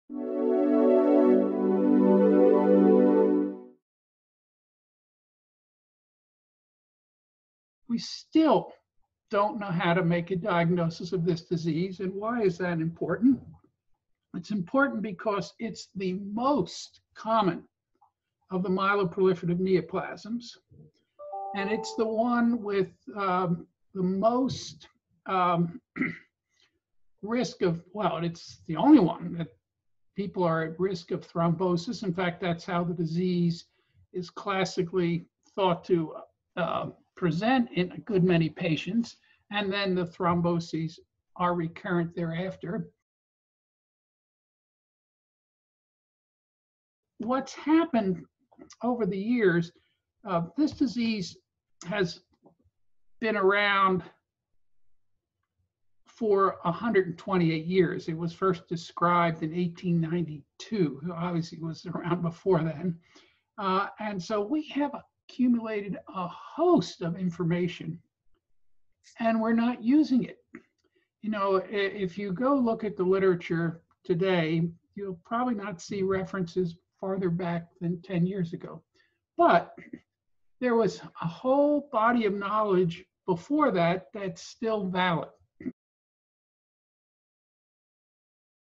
Dr. Spivak on the Prevalence of Polycythemia Vera
Jerry Le Pow Spivak, MD, professor of medicine and oncology, Johns Hopkins University School of Medicine, director, Center for the Chronic Myeloproliferative Disorders, Johns Hopkins Medicine, discusses the prevalence of polycythemia vera.